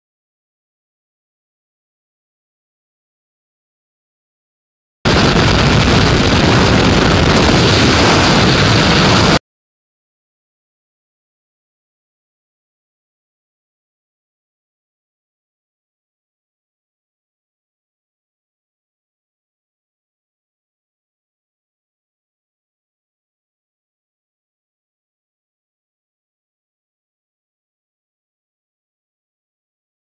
Super Touch
An amusing section of the Round 5 jump at the 1st 2012 UKSL at Peterlee by Team Elements.  Video taken by Mal Smith.